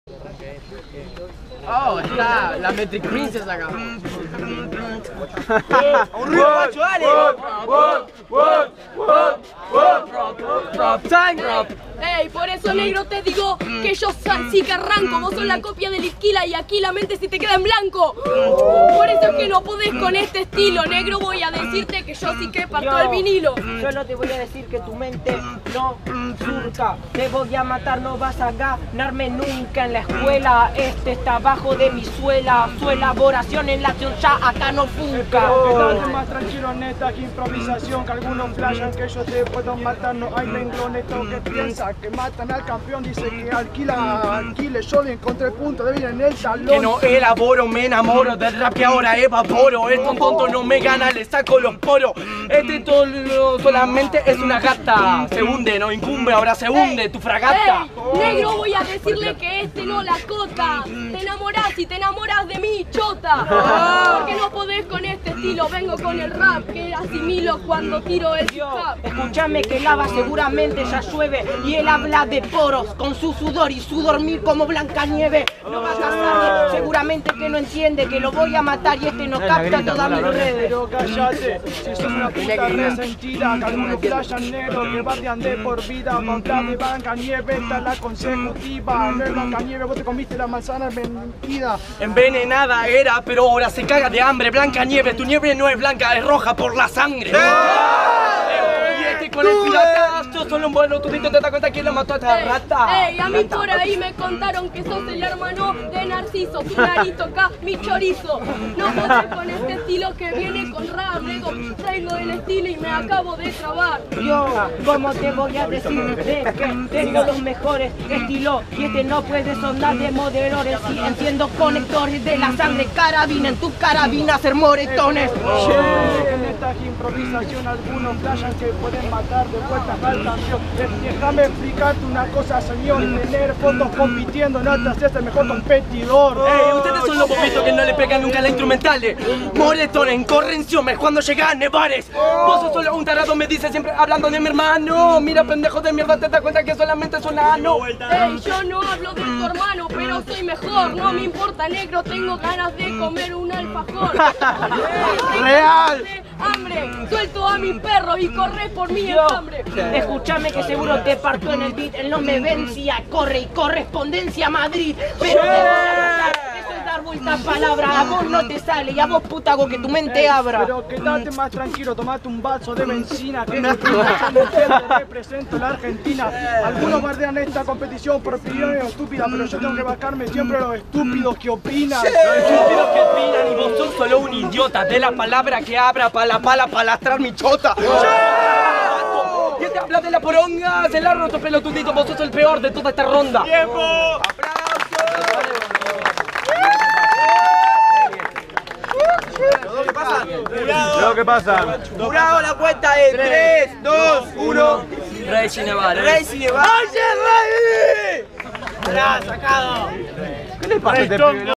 Reiz vs Druff vs Nevares vs Fran-c || 8vos || PoliFree Fecha 3 (Torneo Verano 2017)
Fecha del PoliFree de Verano 2017. Competencia de Freestyle modalidad 1 vs 1,

La compe se hace todos los sabados de este verano en la Plaza Lopes, Rosario.